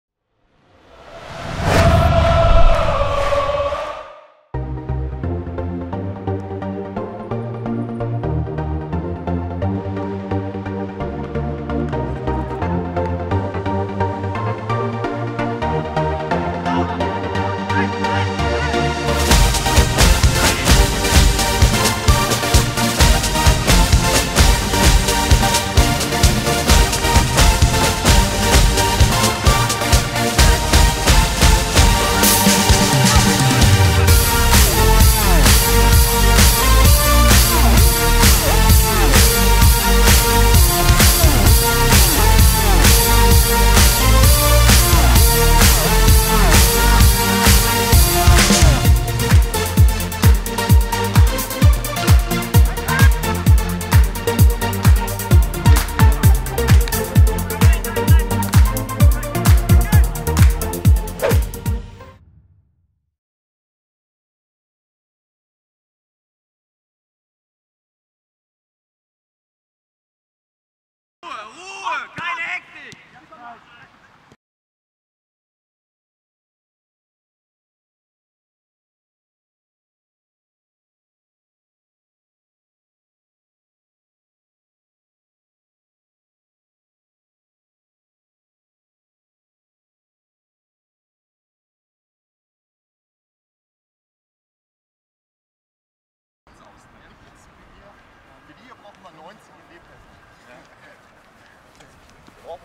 Darmstadt 98 | Die Neuzugänge im Fokus
All eyes on the new guys!

Wir haben unsere Neuzugänge bei den beiden heutigen Einheiten mal etwas genauer ins Visier genommen 😉